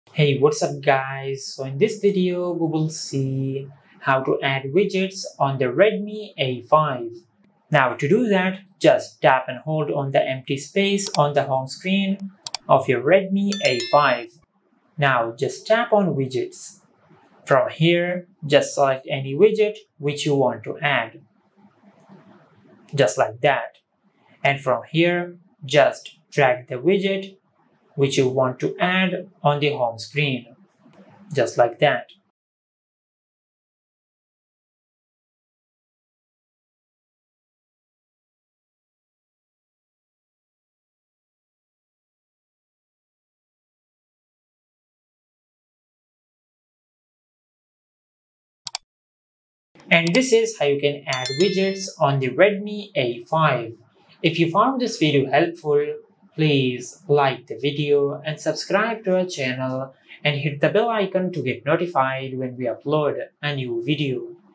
How to Add Widget on Homescreen in Redmi A5 - Widget Settings
How you can set widget on homescreen in Redmi a5. If you want to add widget on homescreen or find widget settings in Redmi a5, follow these steps.